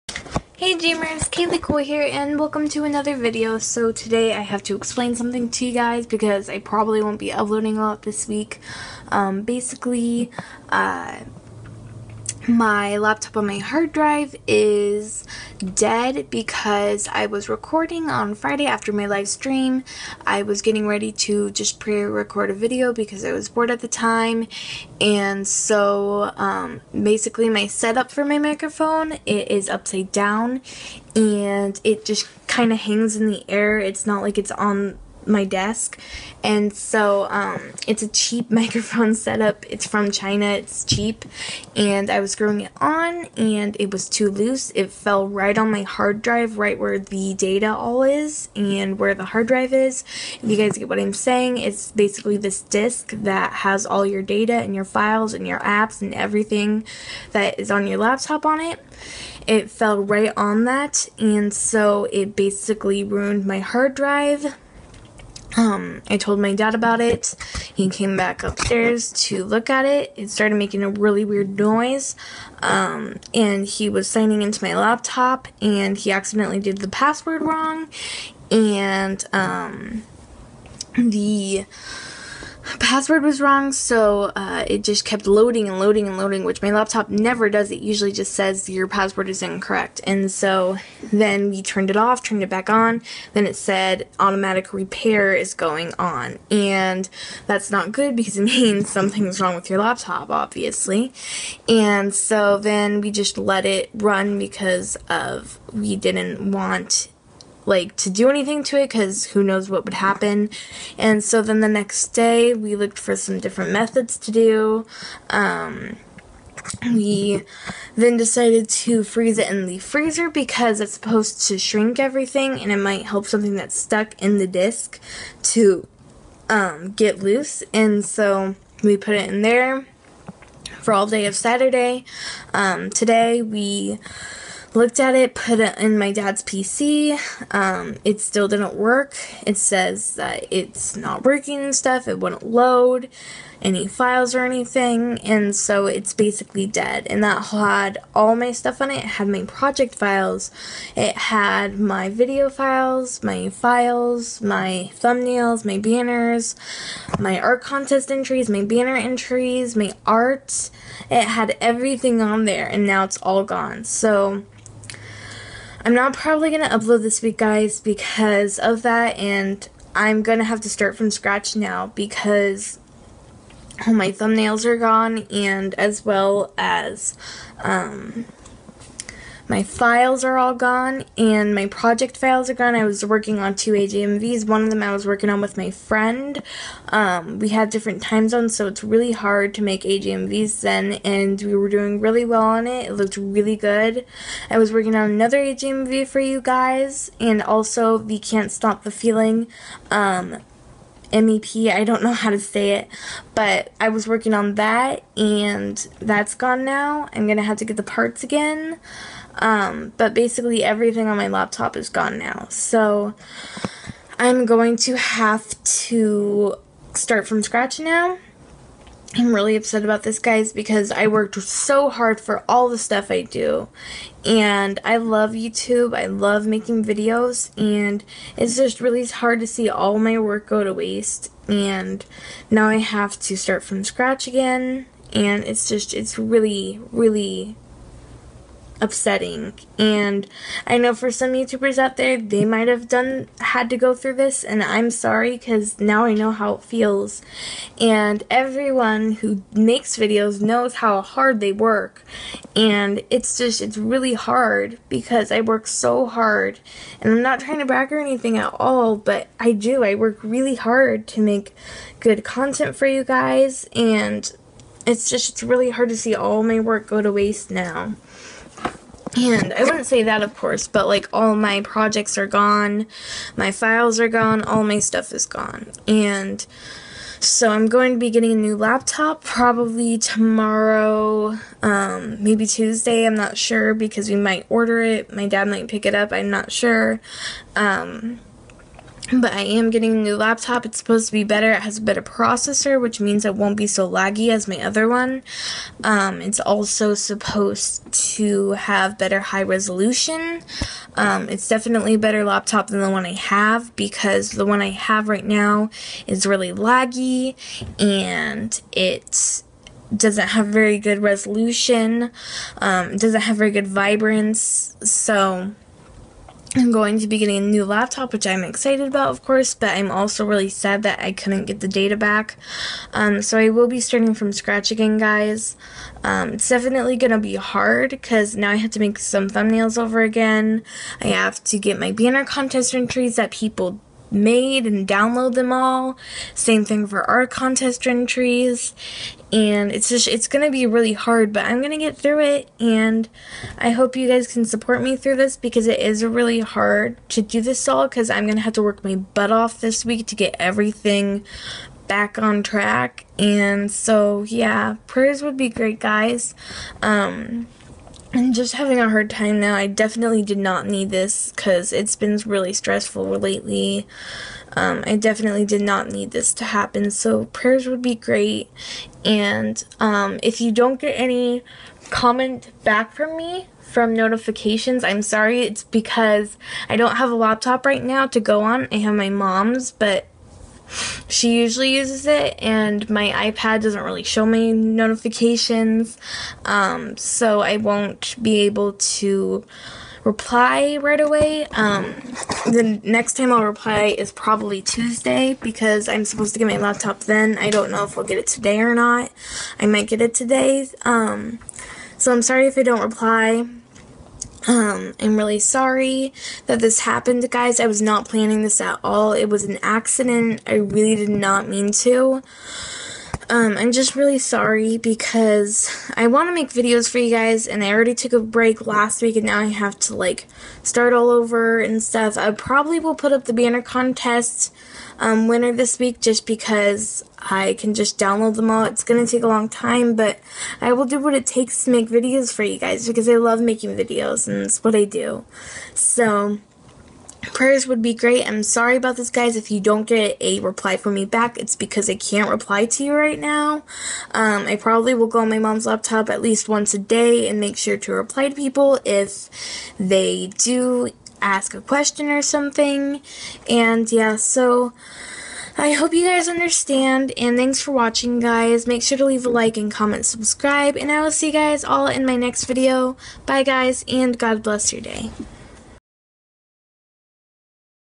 Please watch this! *IMPORTANT*
Hey jammers! I am really sorry about this all. I didn't mean for it to happen, it was an accident. I hope you all understand, it's going to be really hard starting from scratch. But I am glad I am getting a new laptop, which will be nice since mine isn't that great. Also, shoutout to my awesome dad, he helped me try to get my data/hard drive back, which was really nice of him to do since it is Father's Day weekend. Happy Fathers Day to my wonderful dad.